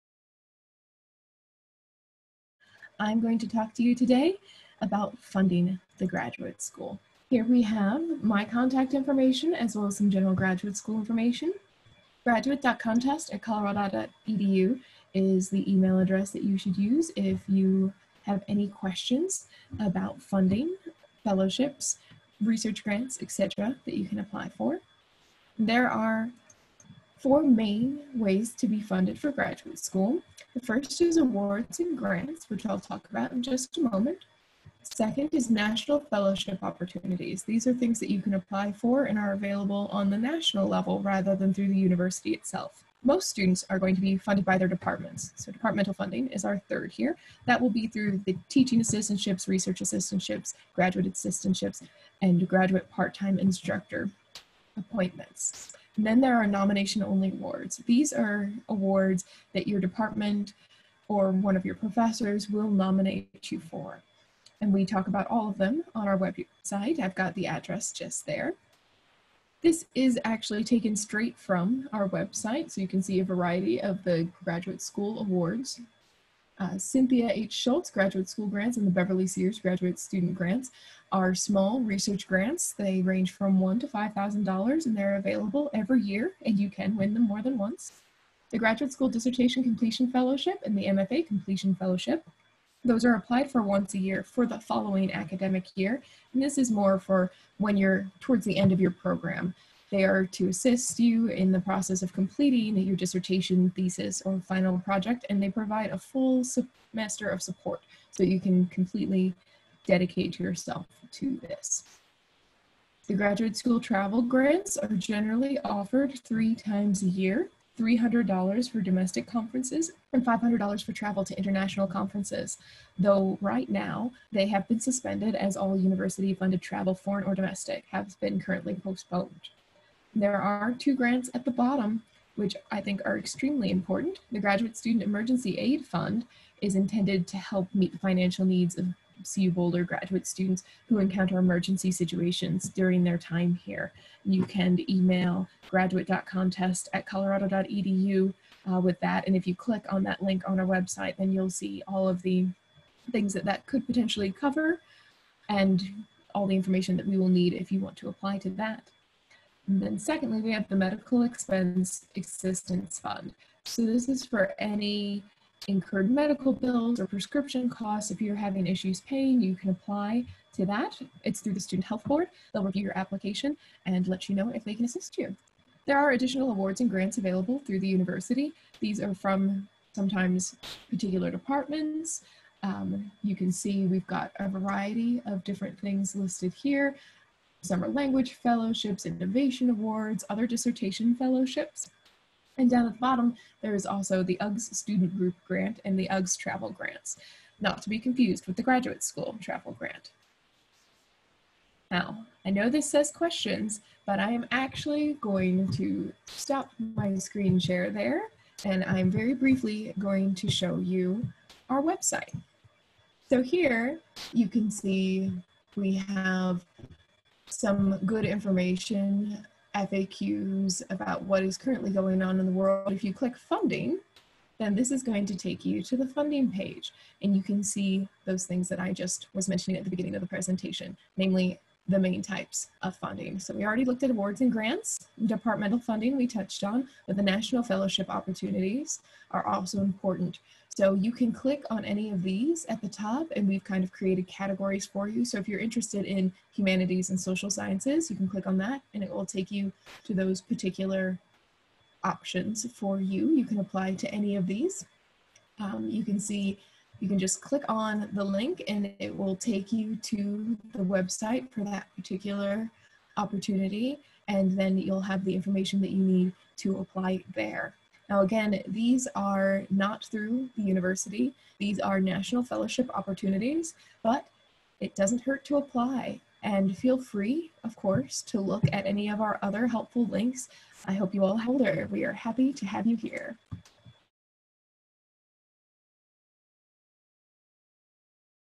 Graduate School Funding | CU Boulder
Learn more about graduate school funding, fellowships, research grants and awards at CU Boulder.